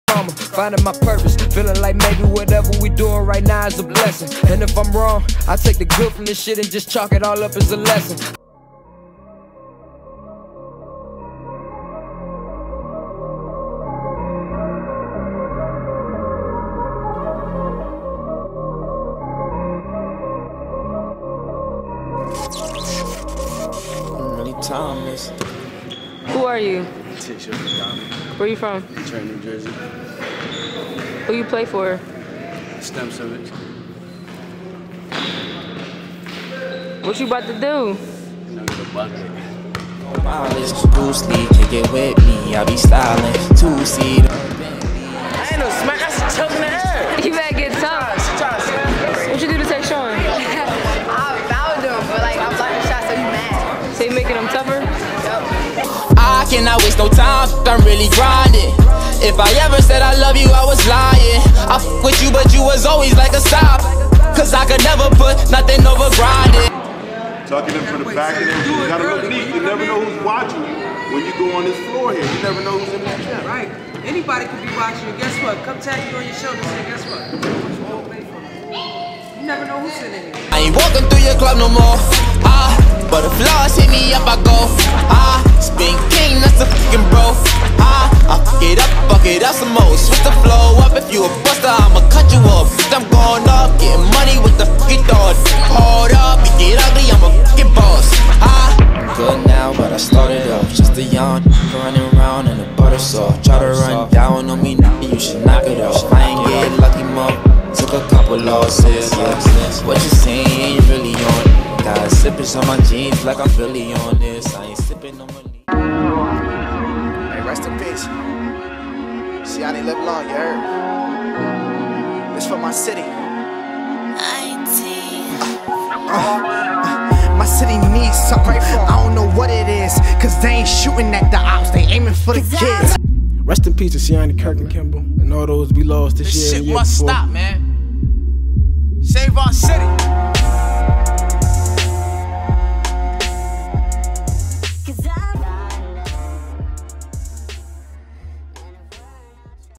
La'Keisha Sutton Fan Favorite Diary : featuring Tashawn McDonald
This video features 16 year Tashawn McDonald who plays basketball at Stem Civics High School in Trenton, NJ. Tashawn wants to make a name for himself and become a "Fan Favorite" too in his basketball career.

I do 1 on 1 basketball player development sessions with Tashawn to add some @fanfavoritemoves to his game. Excited to see the player he becomes if he continues working hard. We will capture it all, his life on and off the court.